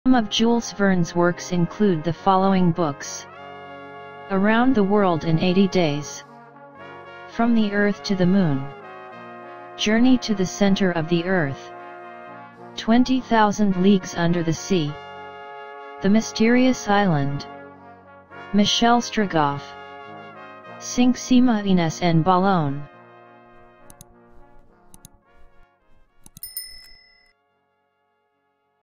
Jules Verne's works
Jules Gabriel Verne was born in Nantes, Pays de la Loire, France on February 8, 1828.

He was a French novelist, poet and playwright. His works include best-selling adventure novels

Verne's largest body of work is the Voyages extraordinaires series, which includes all of his novels except for the two rejected manuscripts Paris in the Twentieth Century and Backwards to Britain (published posthumously in 1994 and 1989, respectively) and for projects left unfinished at his death (many of which would be posthumously adapted or rewritten for publication by his son Michel).
Verne also wrote many plays, poems, song texts, operetta libretti, and short stories, as well as a variety of essays and miscellaneous non-fiction

Around the World in Eighty Days‎
From the Earth to the Moon‎
Journey to the Center of the Earth‎
 Twenty Thousand Leagues Under the Sea‎
The Mysterious Island
 Michel Strogoff
Cinq Semaines En Ballon